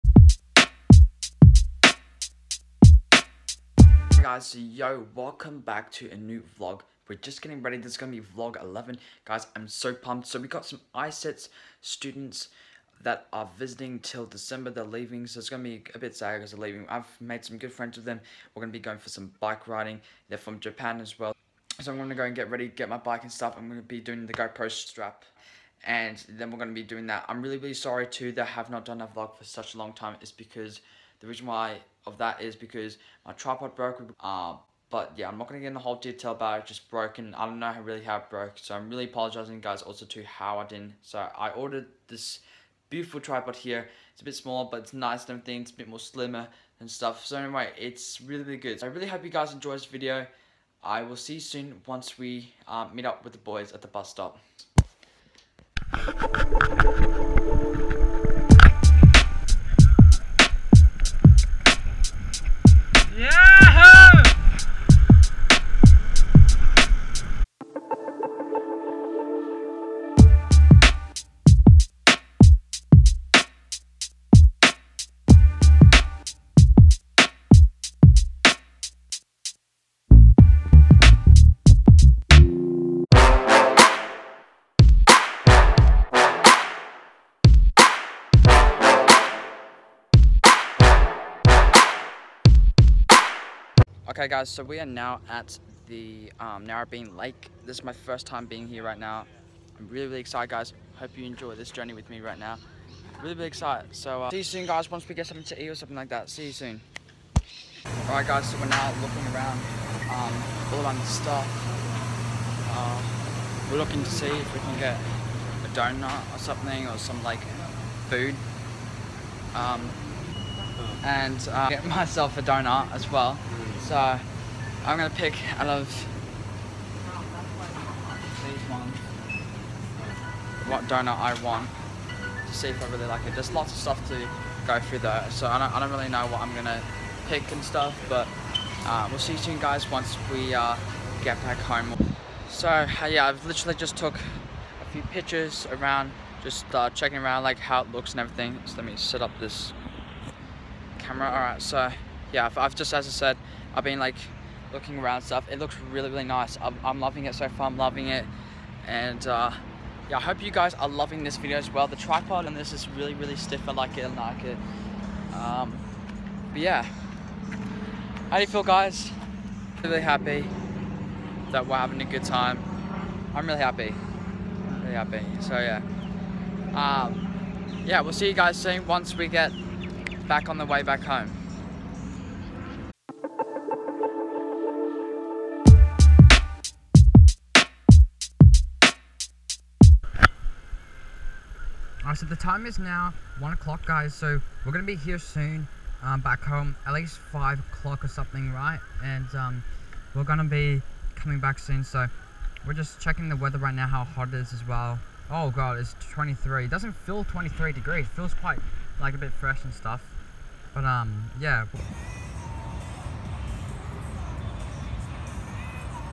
Vlog 11 NARRABEEN
Today was an amazing experience with my Japanese friends. we rode all the way to narrabeen hope you guys enjoyed